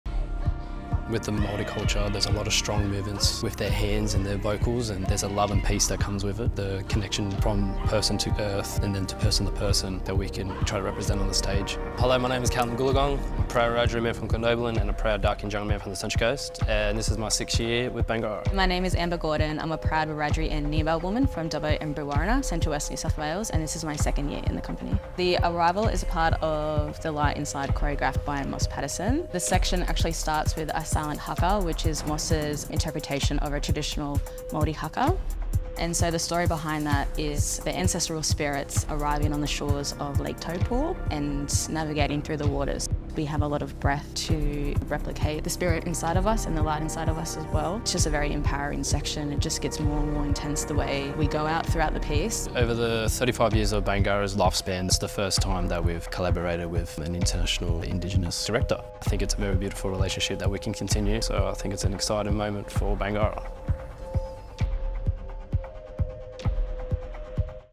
The Arrival | Horizon 2024
Dancers Kallum Goolagong and Amberlilly Gordon describe what it’s like to learn about the Māori culture and perform The Arrival, a section within Horizon by Moss Te Ururangi Patterson.

In our first cross-cultural collaboration, the dancers embody the resilience of the people of Tokaanu and Ngāti Tūwharetoa on the central north island of Aotearoa.

Join us for Horizon and explore the cultural forces that bind us together, across oceans and eons.

11 June - 13 July 2024 | Gadigal Country, Sydney Opera House
18 - 20 July 2024 | Ngunnawal Country, Canberra Theatre Centre
7 - 17 August 2024 | Meanjin,  Queensland Performing Arts Centre
28 August - 7 September 2024 | Wurundjeri Country, Arts Centre Melbourne

Video by Cass Eipper